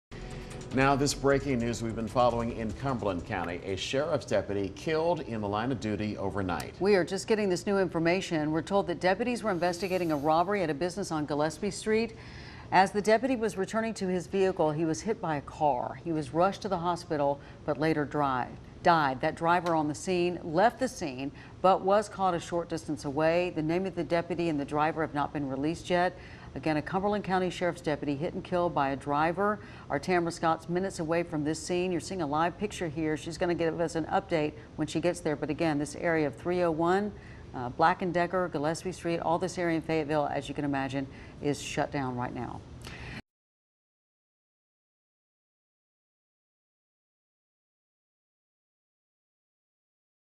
Cumberland County deputy killed after being hit by vehicle, suspect in custody
At 2:46 a.m. the deputy, who has not been identified, was investigating a robbery at a business on Gillespie Street on Highway 301 in Fayetteville.